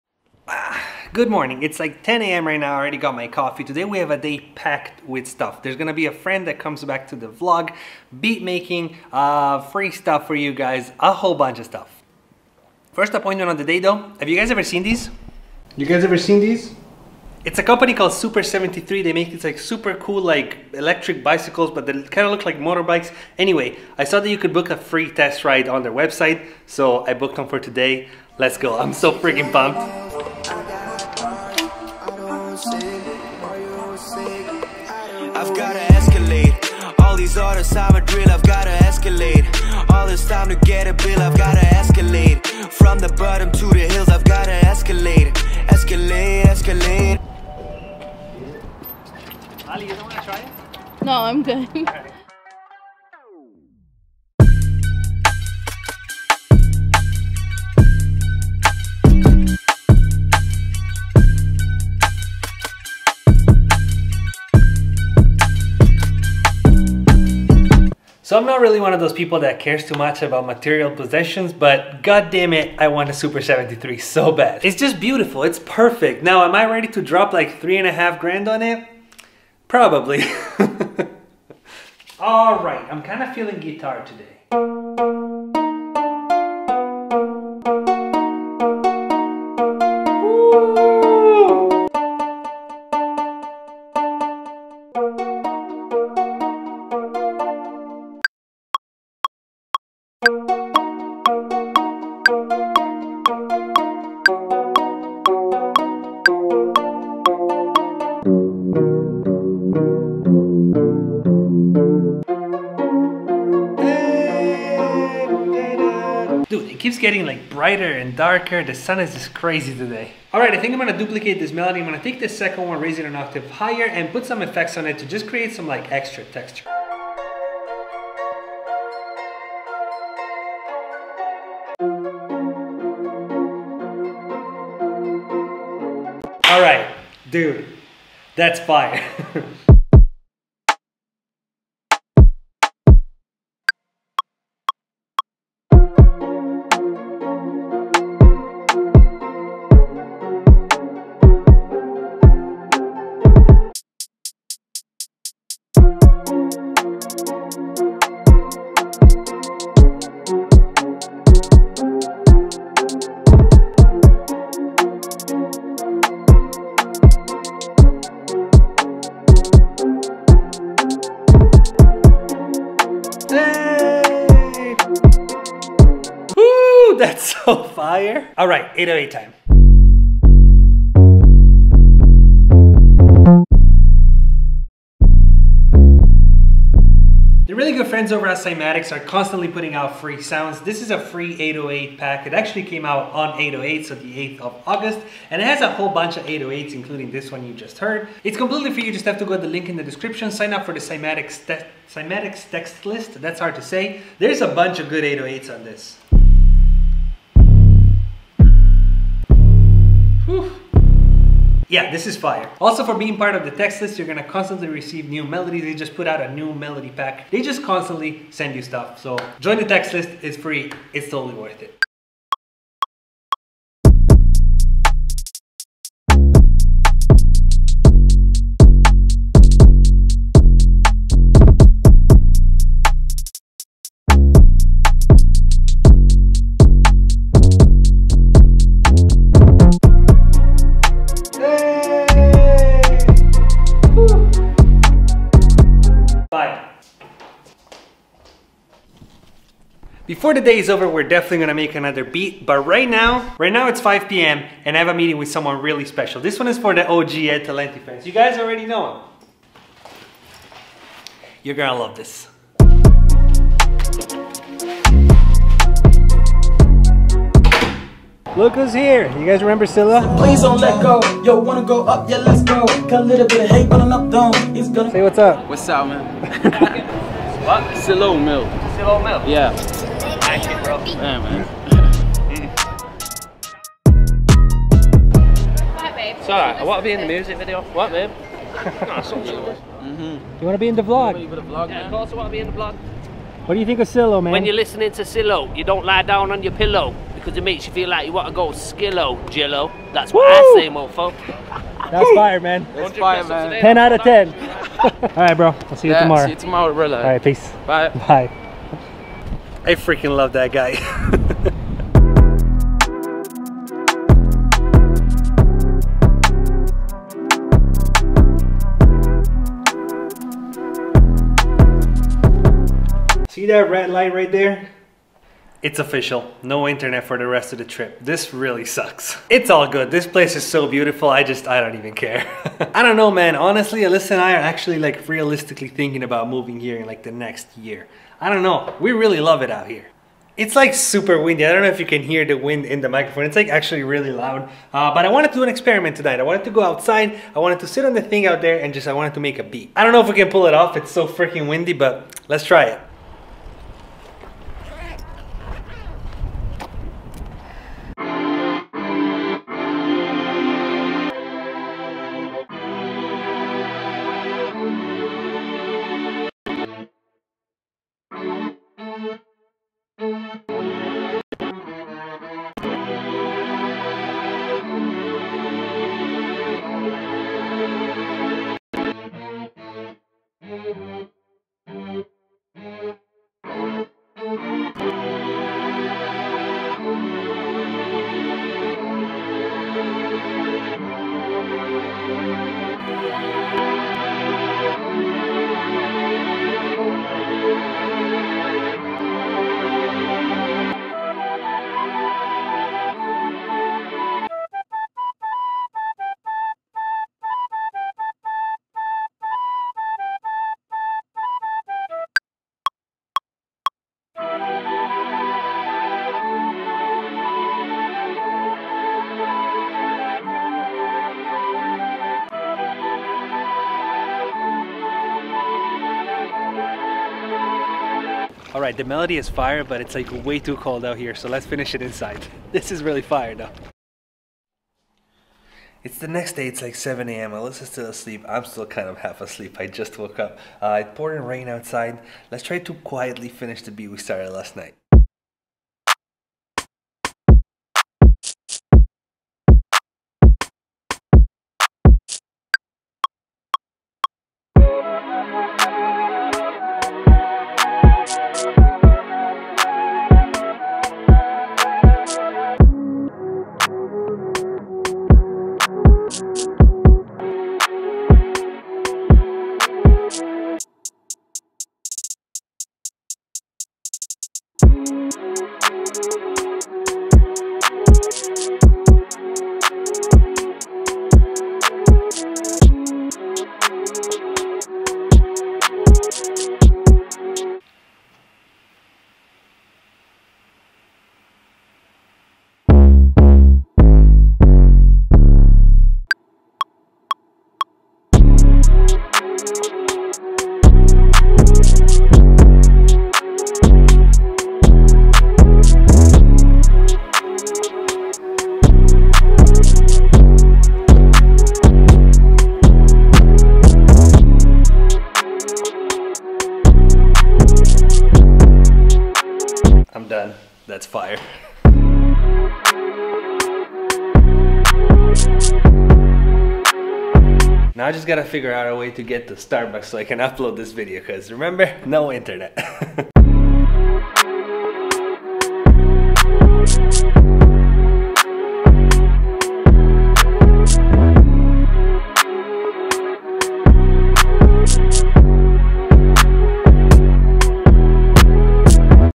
THE BEST *free* SAMPLE PACK for HARD 808s!! (its insane) | music producer vlog
CHAPTERS
00:00 - This costed me $3.500!!
01:21 - First Cookup
04:05 - A very SPECIAL GUEST!!
04:45 - You NEED to watch this! 😂
05:53 - Making a beat ON THE WATER!! (pt.1)
07:12 - Making a beat ON THE WATER!! (pt.2)

[IGNORE THIS]
Made with Ableton Live 10